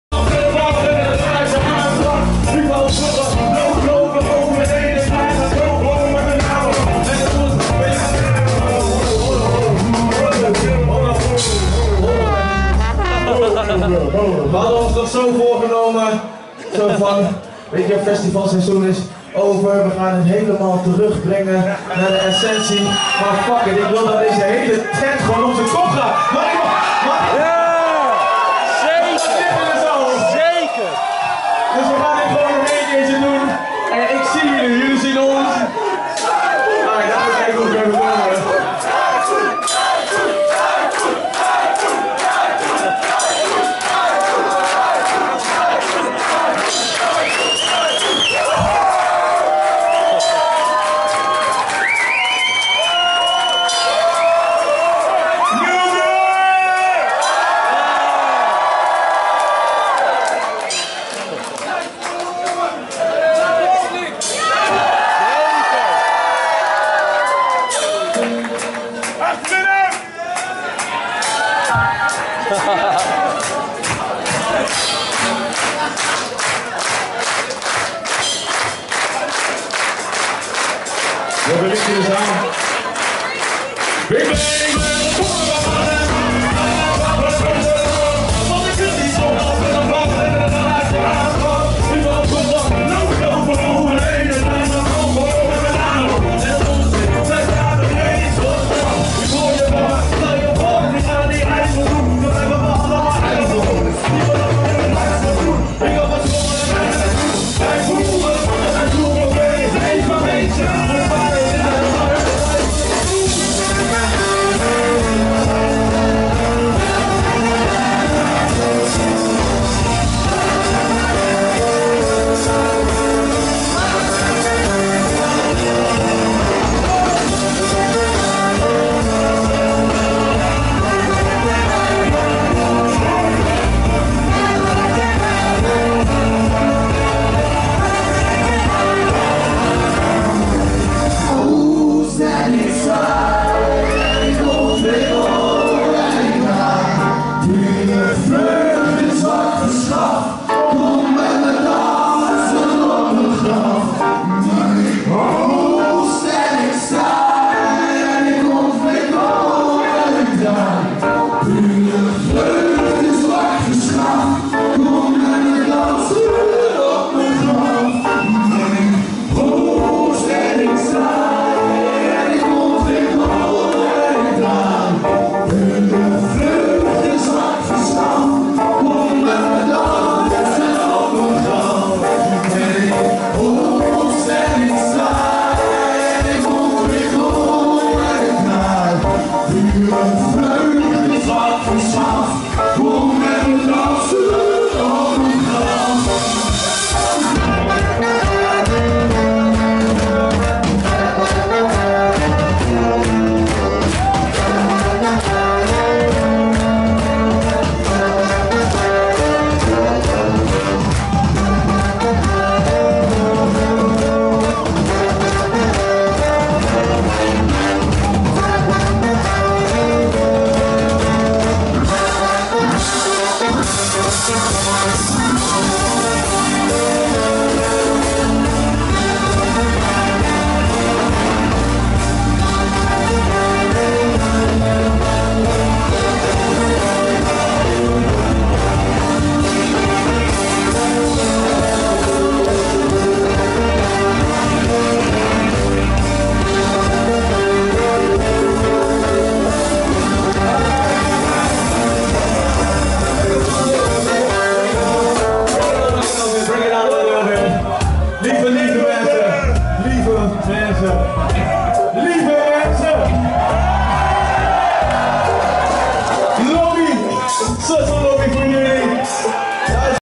Typhoon feest in de Spot Middelburg, clubtour 2014-2015
Typhoon in de Spot in Middelburg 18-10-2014
Clubtour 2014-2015. Check dat! Gegarandeerd feest!